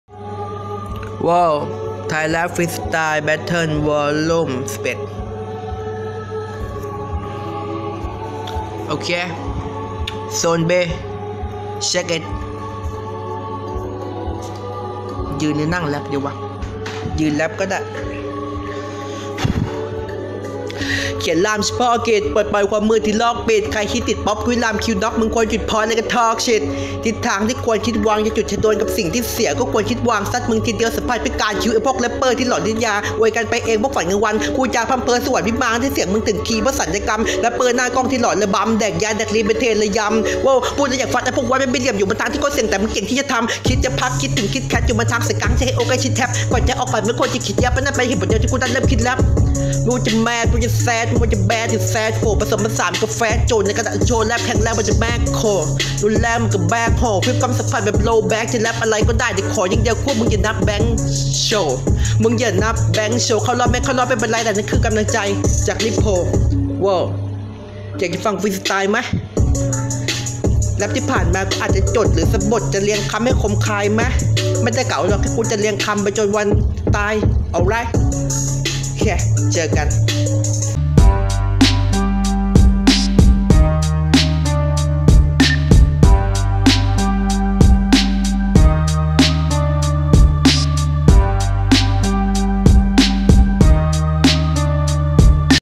Online Audition 11 - ZONE B [Thai Rap Freestyle Battle V.11]
📌📌กติการอบ Online Audition
1. เลือก 1 บีทจาก 5บีทที่ลงไว้ ถ่ายคลิปตัวเองโดยการ
   แบบ LONG TAKE (ไม่ตัดต่อ) พูด AKA ก่อนแร็พ จะจดหรือสดก็ได้ แต่ต้องเป็นเสียงสดเท่านั้น
2. ความยาวคลิป ไม่เกิน 1.30 นาที
3. ไม่จำกัดเนื้อหา (ห้ามด่าพ่อ-แม่ ชาติ ศาสน์ กษัตริย์)
4. อัพโหลดในยูทูป (เปิดไม่สาธารณะ) ส่งลิ้งมาทาง
5. ส่งก่อนวันที่ 20 ธันวาคม 2563 ก่อนเวลา 23:59 น.
6. คัดเลือก 16 สุดท้าย เข้าสู่การแข่งขัน Final Stage
7. เงินรางวัล 10,000 บาท พร้อมถ้วยแชมป์

BEAT PROD. For ONLINE AUDITION
- WIZLY
- BD
- NXTSHT

- DIAMONDMOON
- SCREWED​ UP
สถานที่จัดงาน Final Stage ร้าน "Icon Club" พุทธมณฑลสาย 1